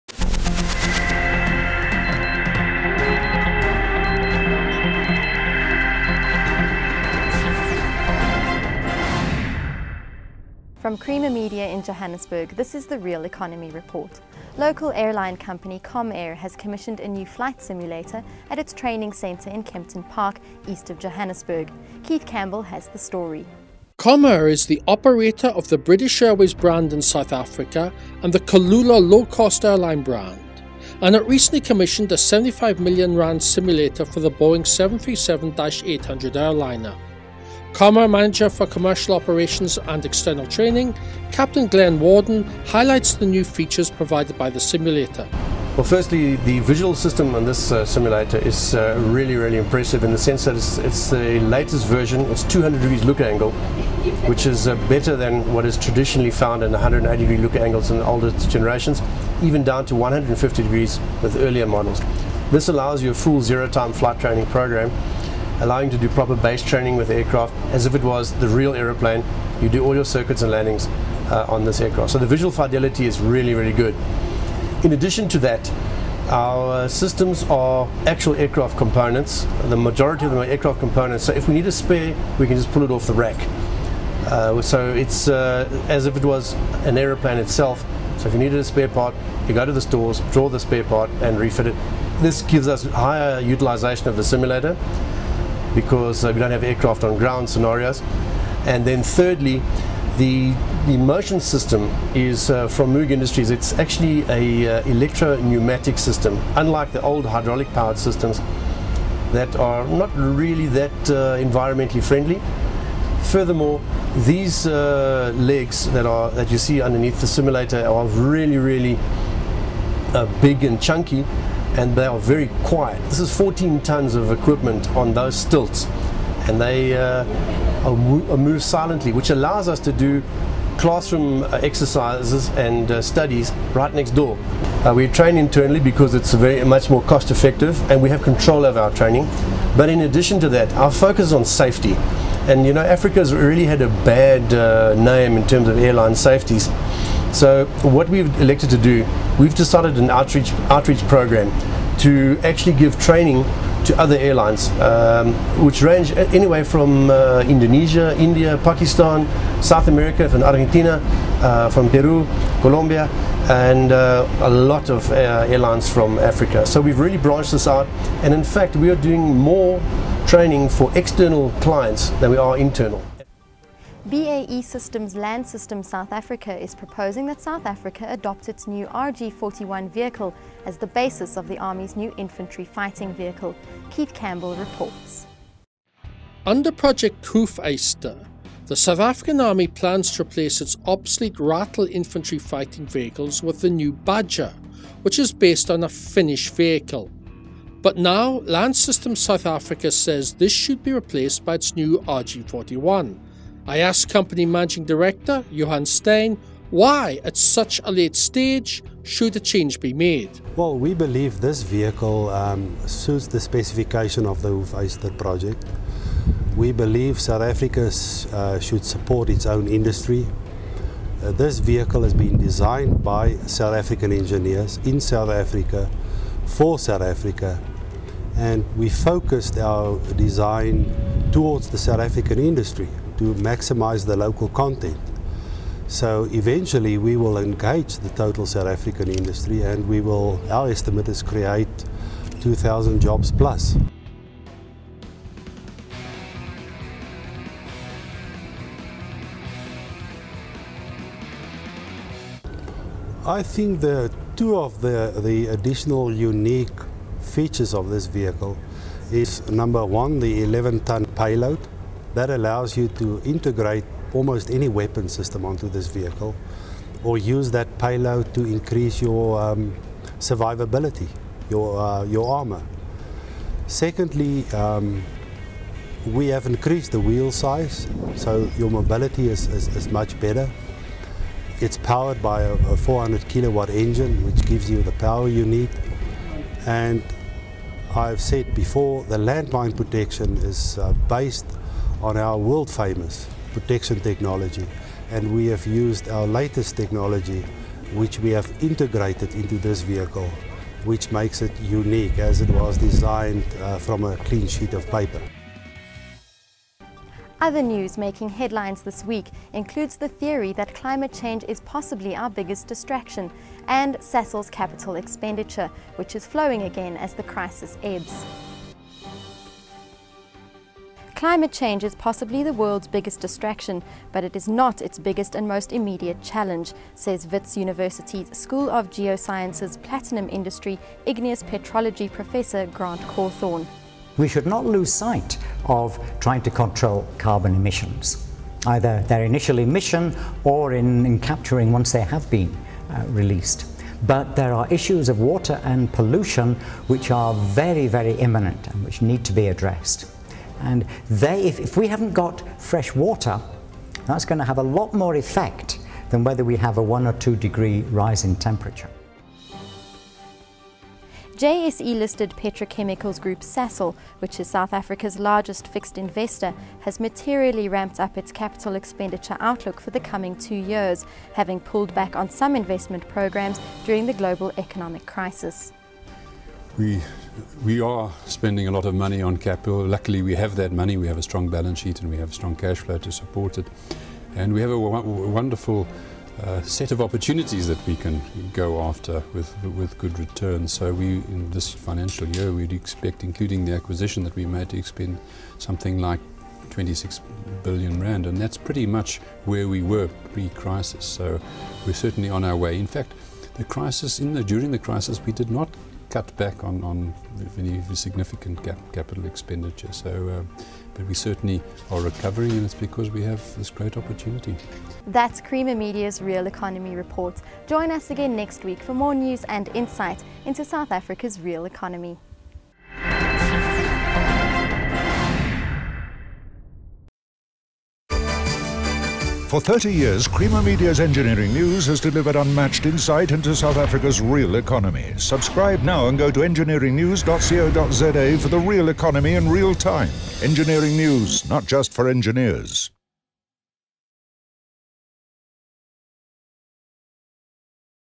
Real Economy Report 144 - Comair
From Creamer Media in Johannesburg, this is the Real Economy Report. Local airline company Comair has commissioned a new flight simulator at its training centre in Kempton Park, east of Johannesburg. Keith Campbell has the story.